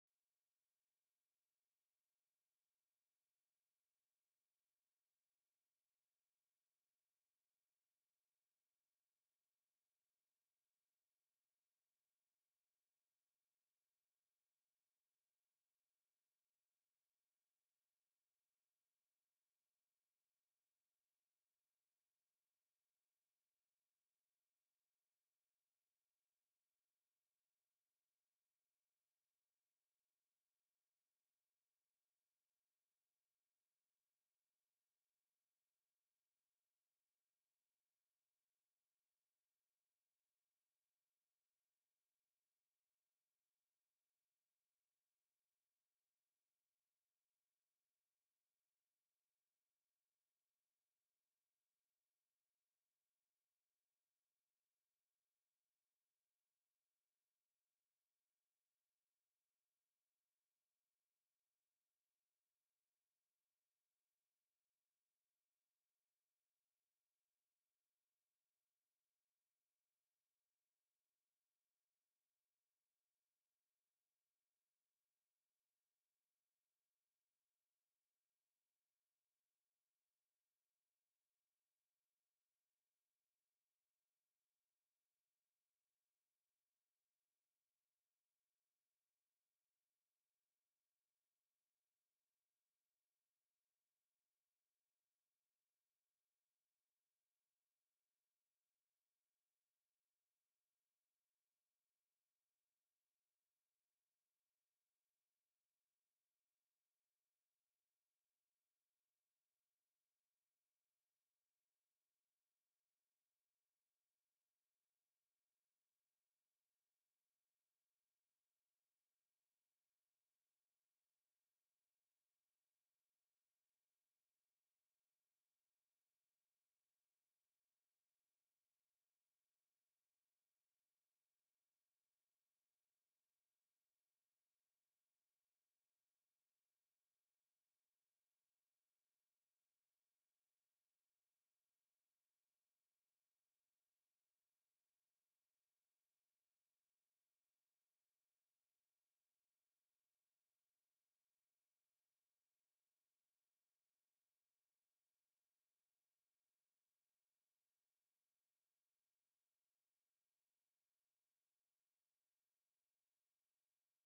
Rovio roaming outside - using rovio commander
A rovio point of view video.  I drove it outside the house starting from the garage, instead of the keybaord, a remote control was used to drive the rovio.